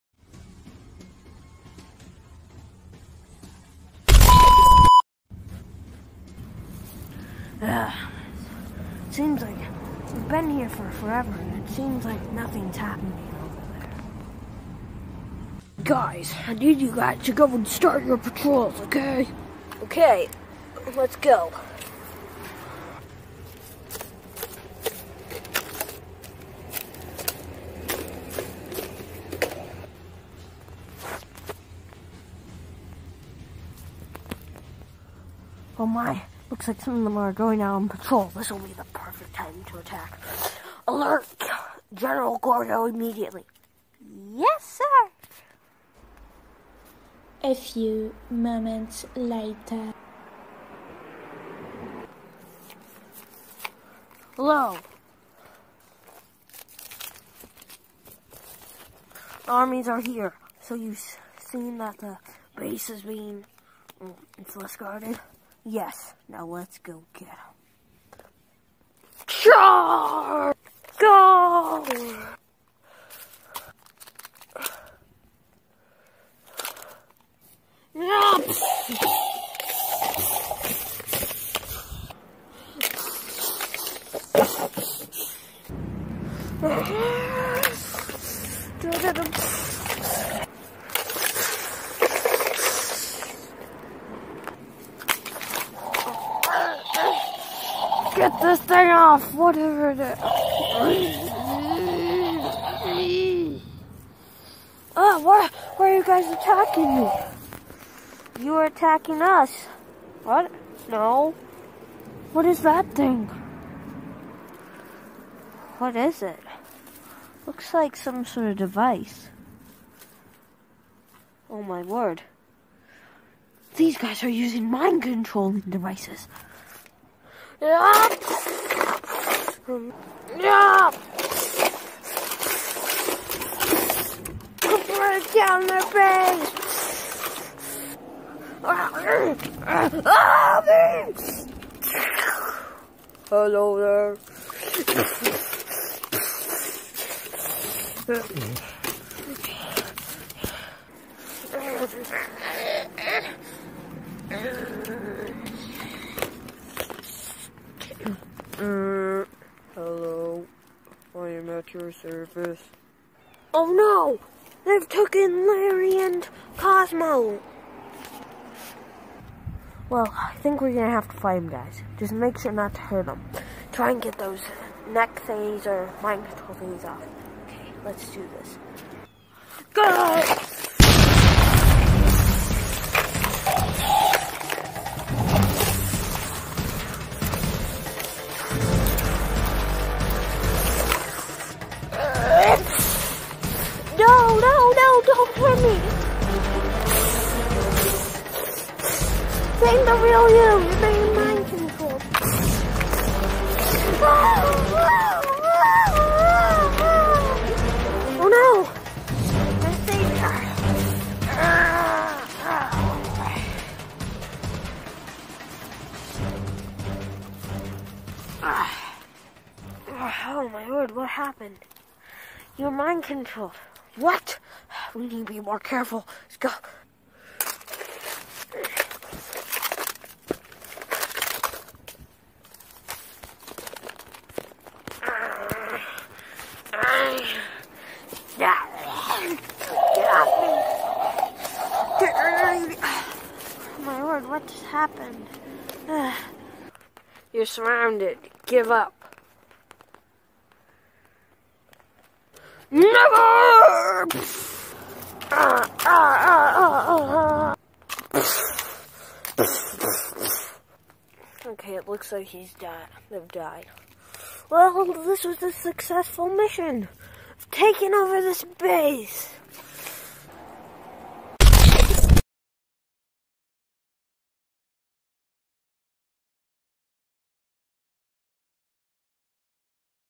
The Forest Base Attack | A Dodo At War
Top Secret recording of the attack we launched on the forest base.
Over and out General Gorgo.